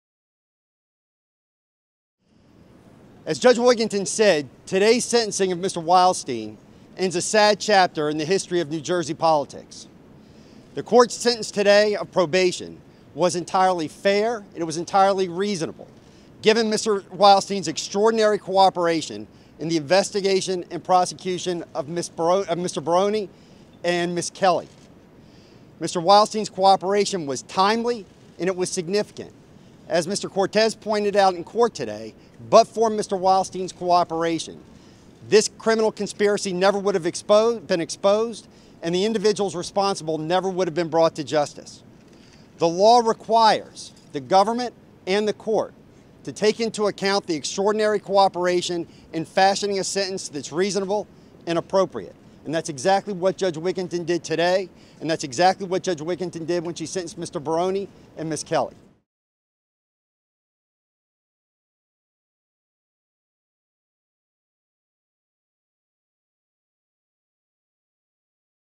Bridgegate: Wildstein receives 3 years probation, 500 hours community service
Bridgegate mastermind, David Wildstein, who plead guilty and gave testimony that lead to the conviction of Bridget Anne Kelly and Paul Baroni, will avoid prison and was sentenced to three years of probation, 500 hours of community service, and nearly $25,000 in fines.